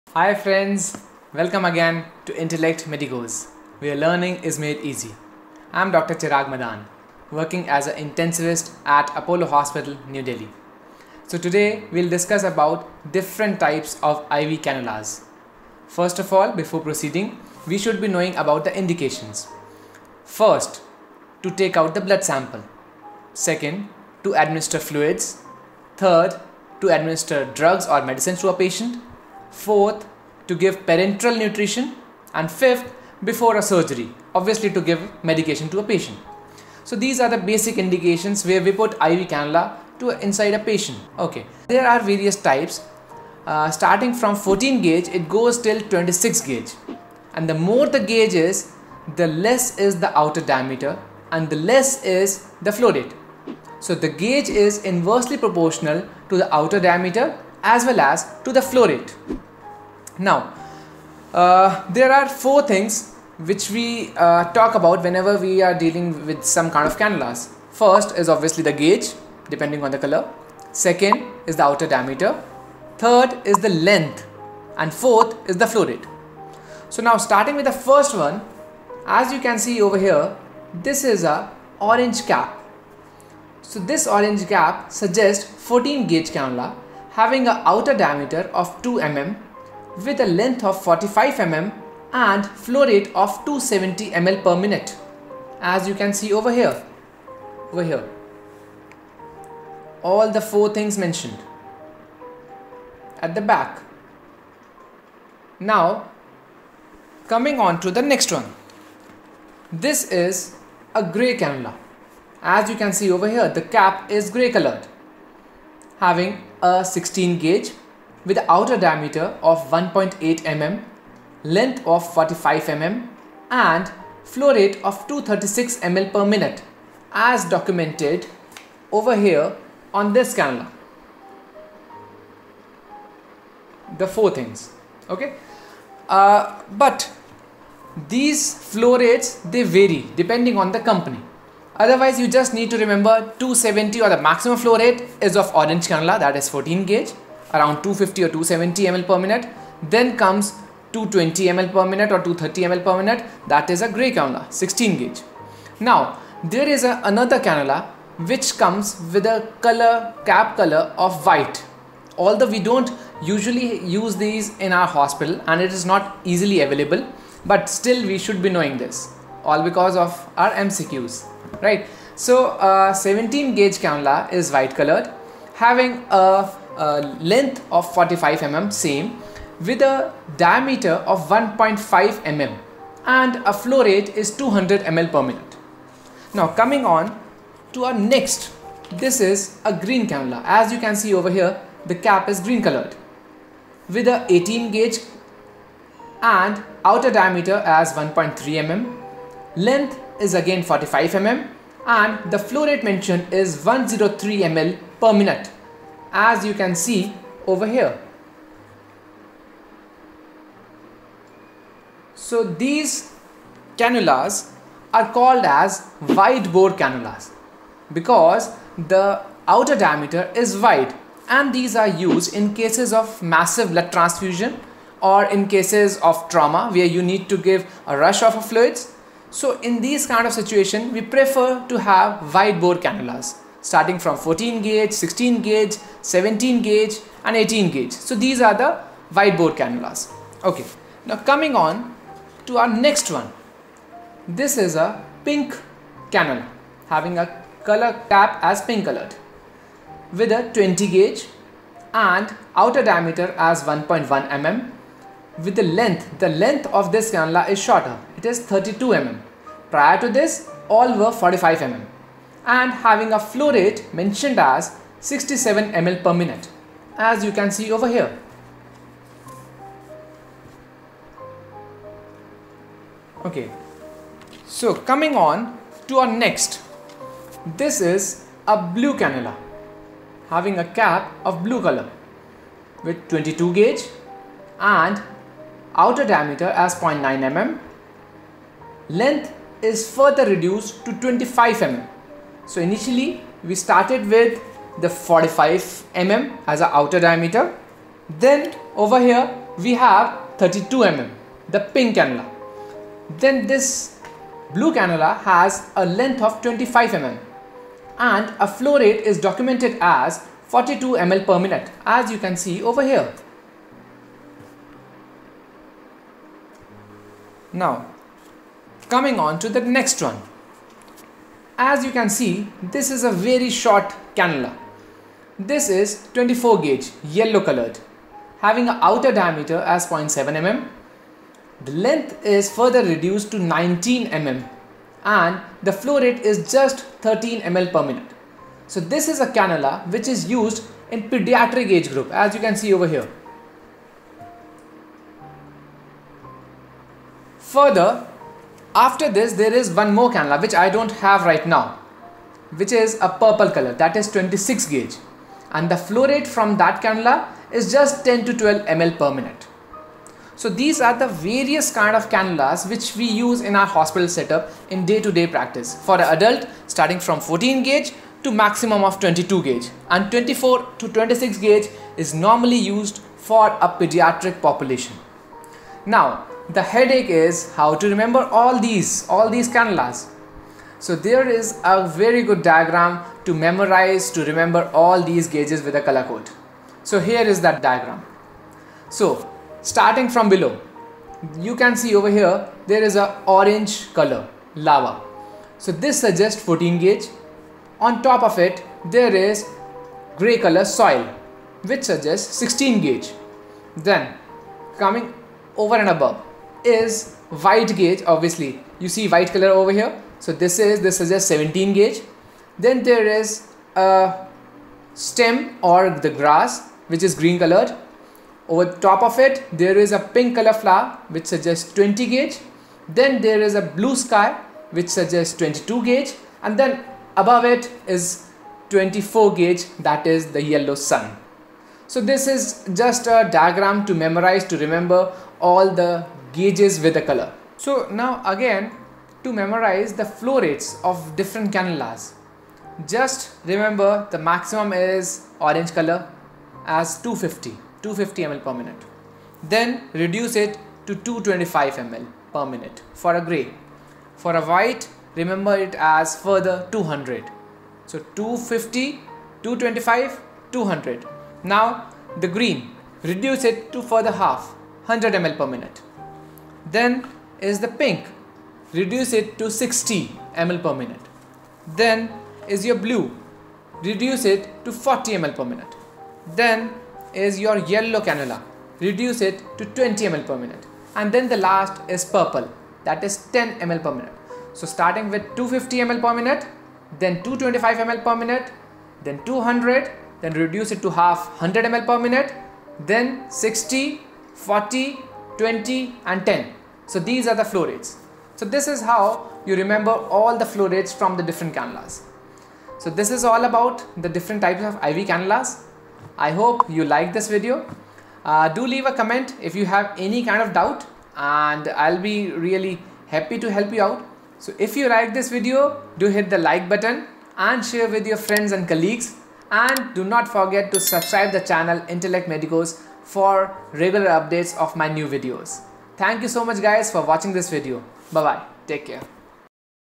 IV CANNULA: Color, Size, Length, Flow rate (mnemonic inside)
Hi guys. In this video, we have explained the different types of cannulas which we use commonly in our hospital setup. This is very important whenever you are managing your patient and also for your various MCQ examinations (for doctors & nurses).

Sometimes it becomes difficult to remember the correct color & the gauge size. To make it an easy task for you, we have given a mnemonic so as to help you memorize the correct color, size & flow rate (do watch this video till the end)!